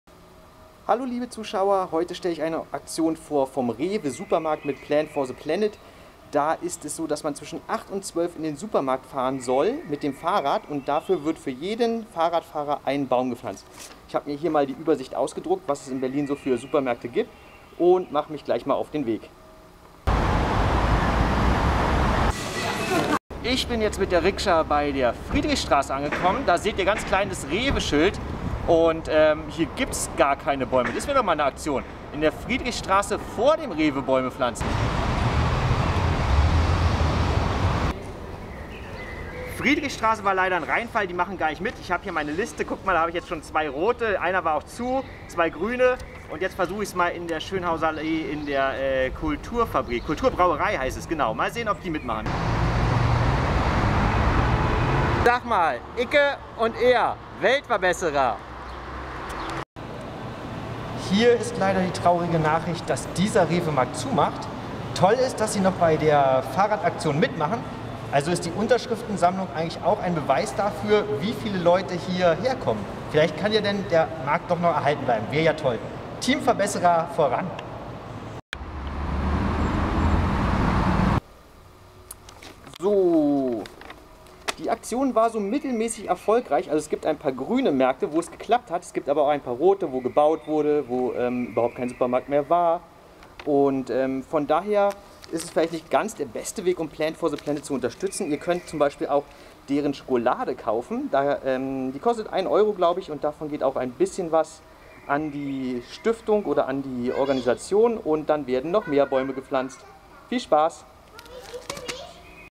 HolK lässt pflanzen: Radeln für Plant for the Planet // HolK.TV Folge 026
Am 8. Juni 2013 fand eine coole Aktion für Plant for the Planet statt. Für jeden, der mit dem Fahrrad zu REWE gekommen ist, soll ein Baum gepflanzt werden. Allerdings war REWE selbst gar nicht so hinterher, möglichst viele Mitmacher zu gewinnen: Nur wer selbst nachgefragt hat, wurde eingetragen, ein Markt hat gar nicht mitgemacht und ein anderer wurde gerade umgebaut. Naja, am Ende konnte ich dann aber doch gut ein Dutzend Einträge "organisieren".